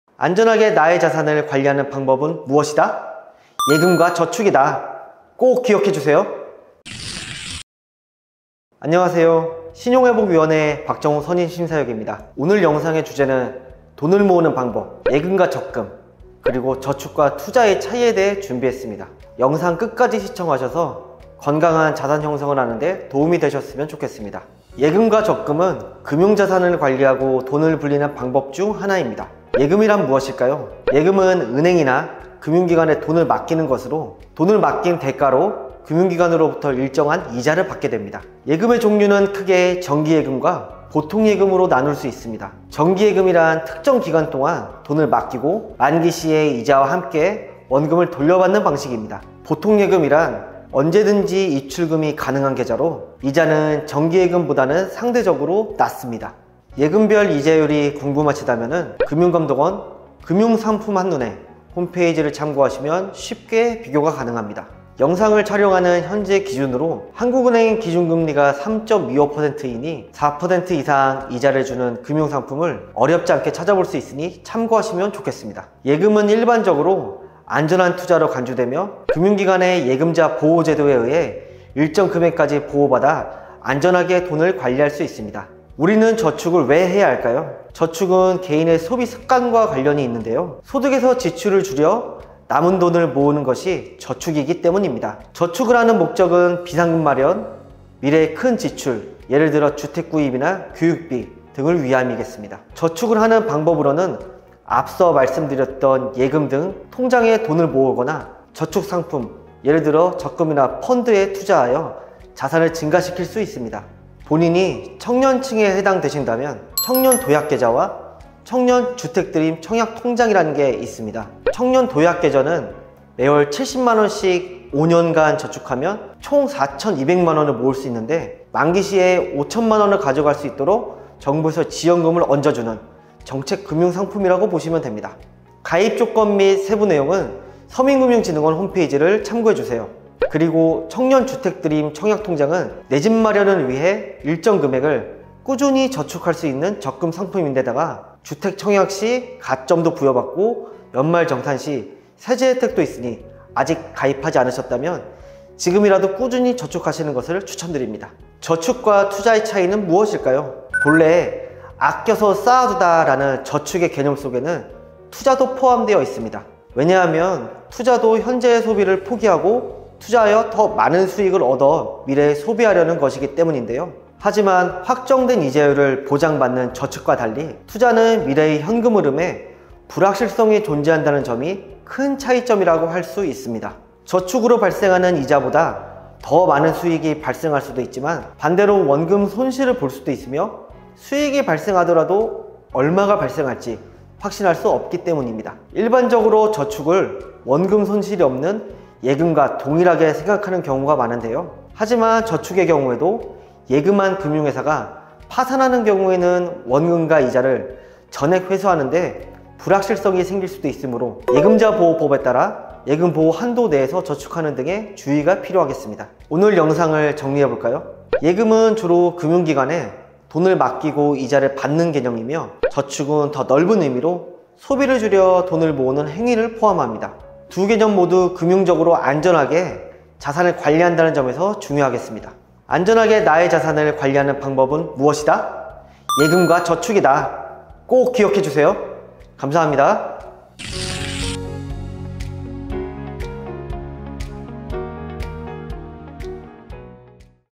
미웠던 고금리가 오히려 반가워지는 안전자산!!
안녕하세요. 신용회복위원회 박정욱 선임심사역입니다. ^^
오늘은 안전자산에 대해서 함께 이야기 나눠보고자 합니다.

투자하면 코인, 주식 등 위험자산을 말씀하시는 경우가 많은데,
현명한 투자자는 본인의 위험선호도와 리스크 관리 능력에 따라 위험자산과 안전자산에 알맞게 배분합니다.

일반적으로 사람들은 저축을 곧 안전자산이라고 생각하시는데요.
대표적인 안전자산에는 무엇이 있고, 저축과는 어떻게 다른지 저와 함께 보시죠!